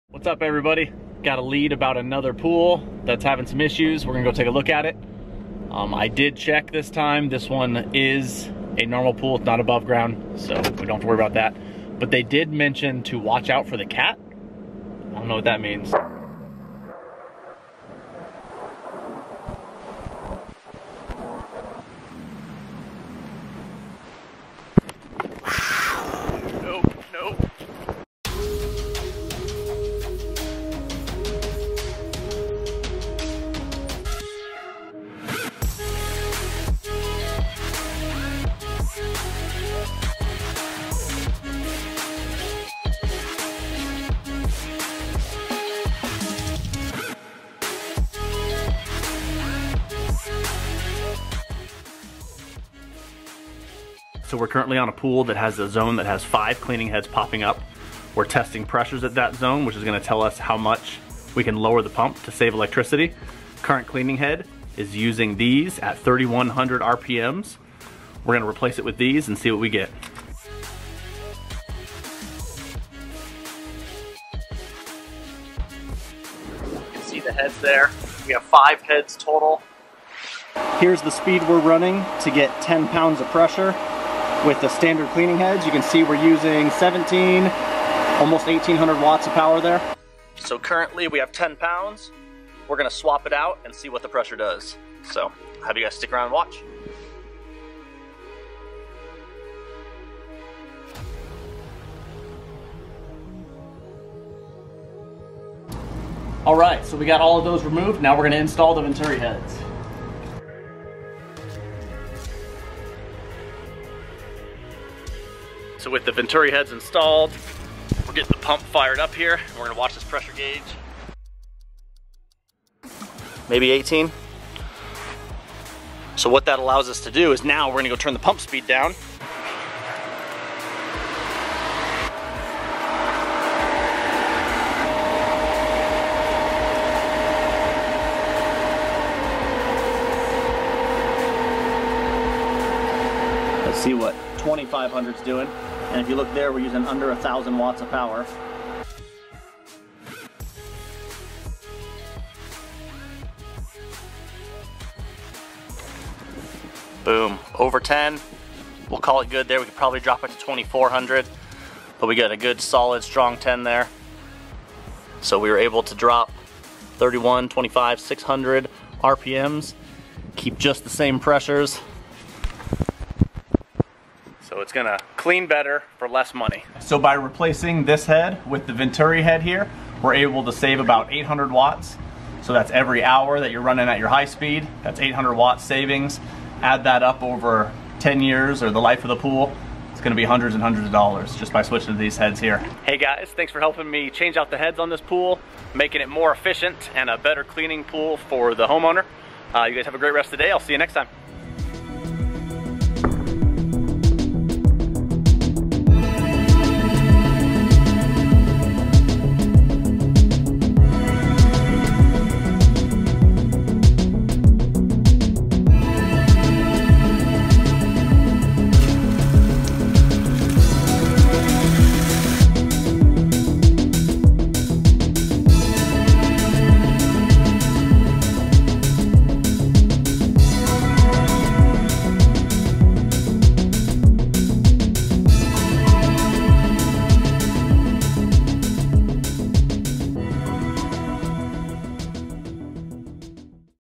Save $100's by Changing This One Item In Your Pool!!
Pools with A&A infloor cleaning systems are able to operate at 40-60% less energy than tradition infloor systems.  I can tell you that all day but watching the video helps you see it happen.

Part Numbers

Head removal tools
G4 Metal Tool: 550272

G4 Plastic Tool: 547508

Venturi Cleaning Heads
G4V White: 565571

G4V Grey: 565554

Pressure Tester
G4 Pressure tester: 548295